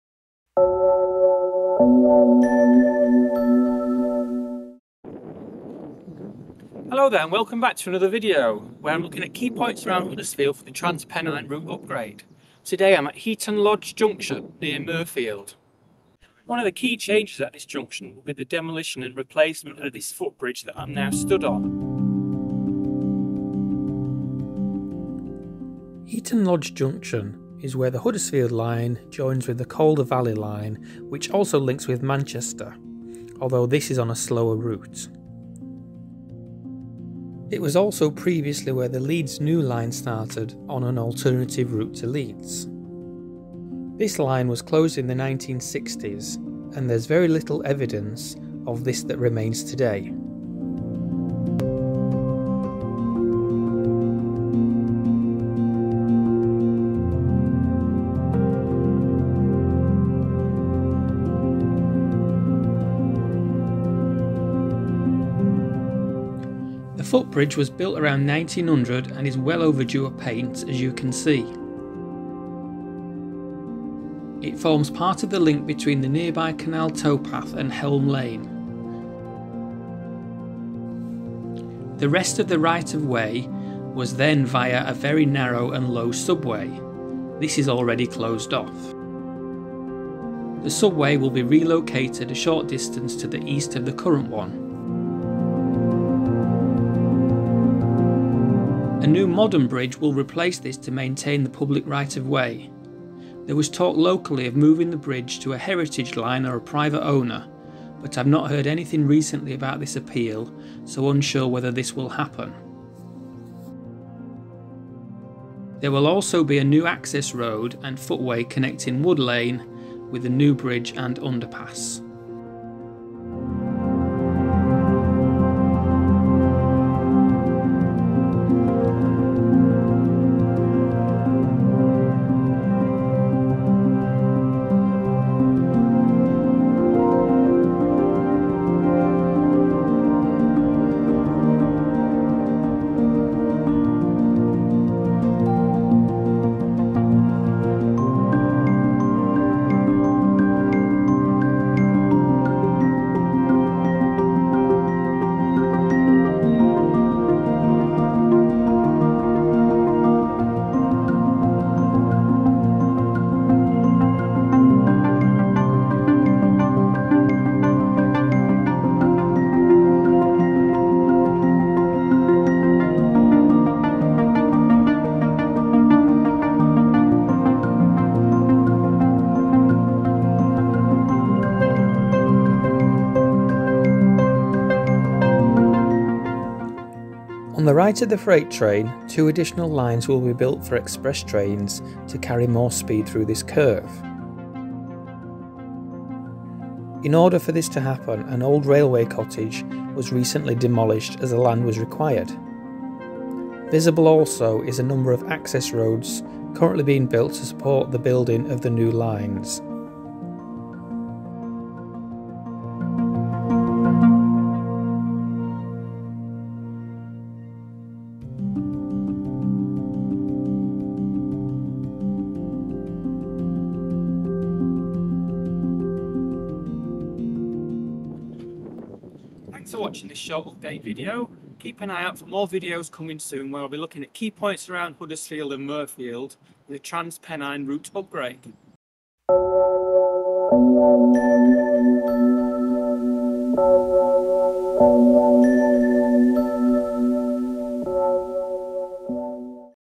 Update by DRONE: Heaton Lodge Junction, Transpennine Route Upgrade
Join me on a new series of videos where I look at the Transpennine Route Upgrade around Huddersfield mainly by drone. In this video I'm looking at the ongoing work around Heaton Lodge Junction where the Huddersfield Line joins with the Calder Valley Line. The existing footbridge will be replaced and two additional lines will also be built to allow express trains to carry more speed through the curve

We get a unique view of the work from 50m up in the air and I explain some of the work currently happening

More videos coming soon for different points around Huddersfield linked to the Transpennine Route Upgrade